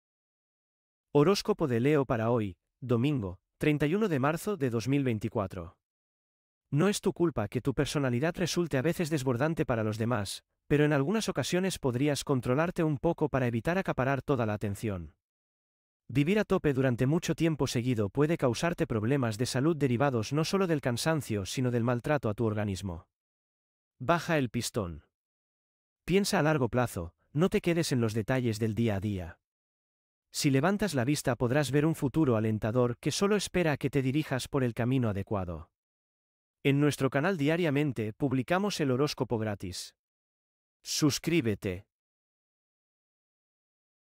HORÓSCOPO DIARIO ⭐❤️🔥 LEO ♌️ DOMINGO, 31 DE MARZO DE 2024
Horóscopo de Leo para hoy, domingo 31 de Marzo de 2024. El horóscopo para hoy: descubre tu futuro gracias a mi horóscopo diario para ti.